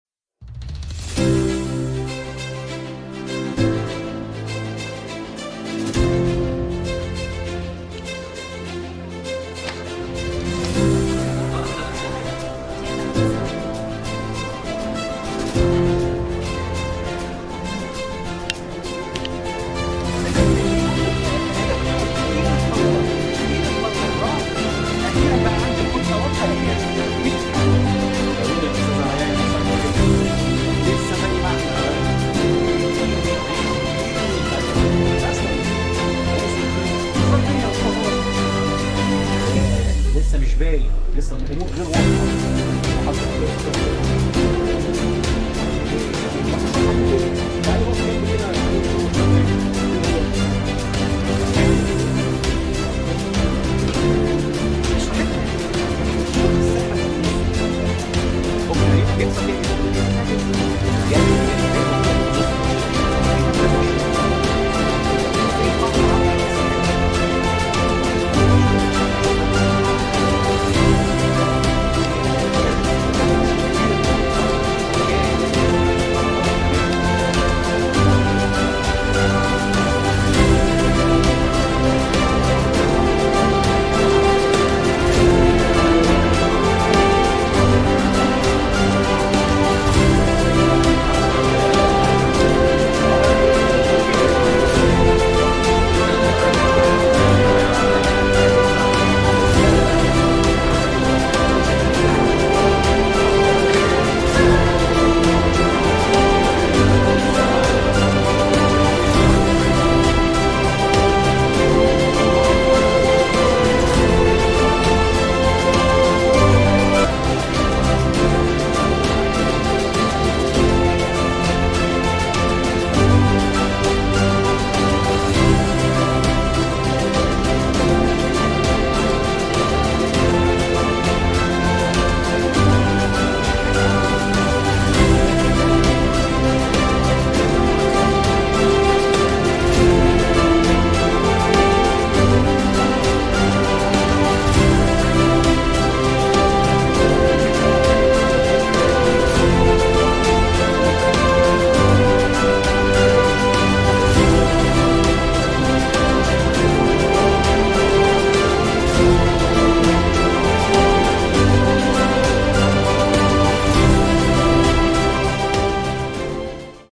The Interactive 4th Level Design Studio, Faculty of Architecture - Beirut Arab University, Lebanon
From the womb of suffering and facing diverse challenges in Lebanon, our students in the Faculty of Architecture, Beirut Arab University, could successfully thrive these difficulties and proved their high quality and dedication in leaning, attendance, and recognizing their architectural characters. The 4th Level Design Studio is an interactive healthy-positive-enthusiastic learning environment. The student Itaf Dessuki documented this 2.45 min. video to capture different activities. This studio was led by the Staff Team (Assoc. Prof. Maged Youssef, Mr. Abd-Al-Razzak Balaa, Ms. Nahed Hamwi, and Ms. Lamis Mantash).
Basic Principle: There is no a failed student, but there is a bad teacher